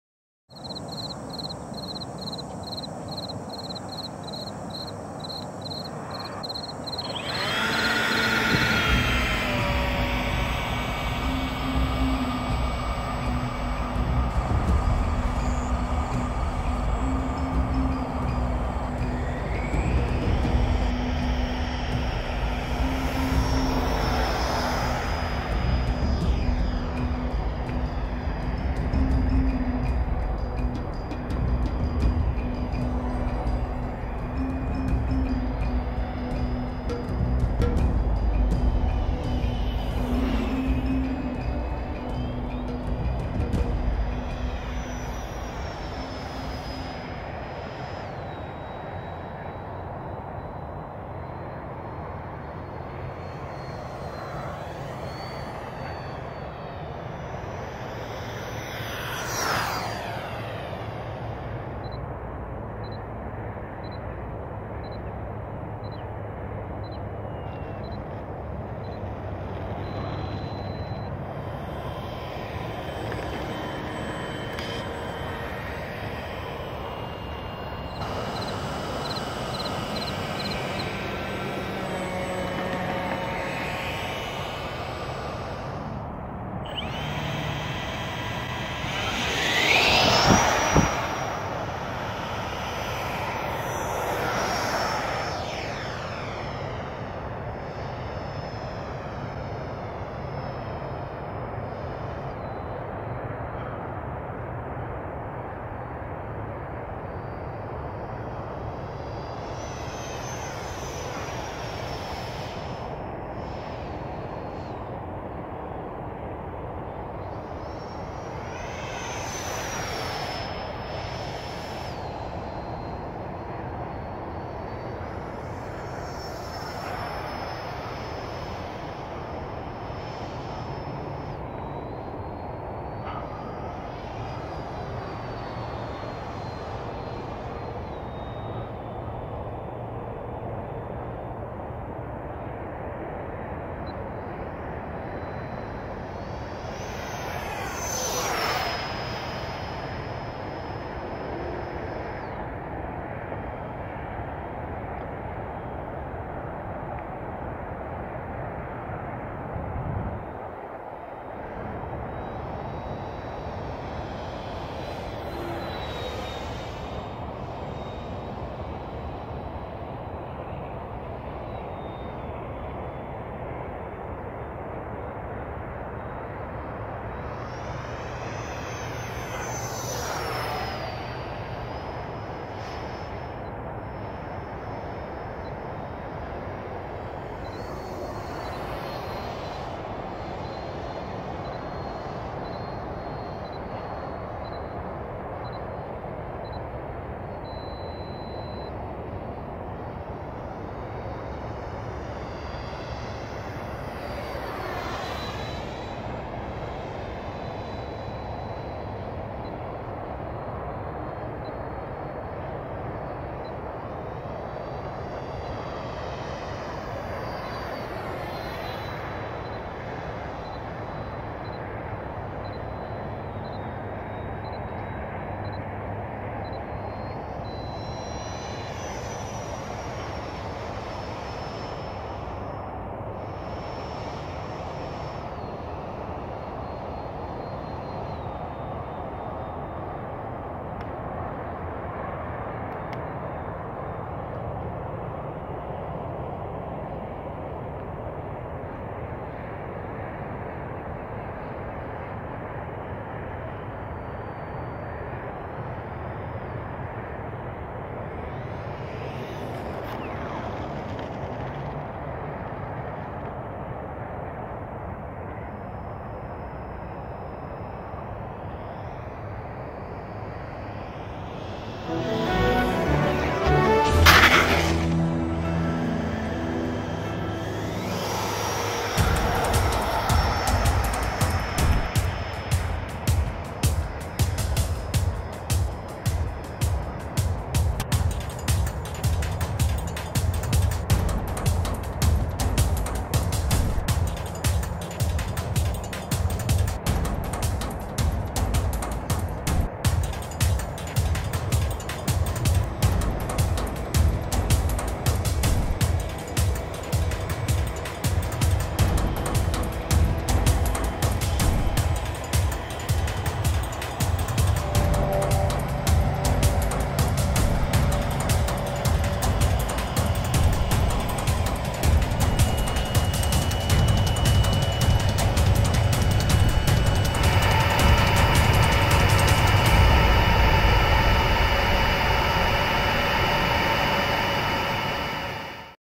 Freewing F-22 flight 1.7.19
Did a crazy spin while trying a knife edge. Guess I over did it on the control surface travel. Wheel knuckle buckle problem needs attention but only happened on moderately sharp turn during landing.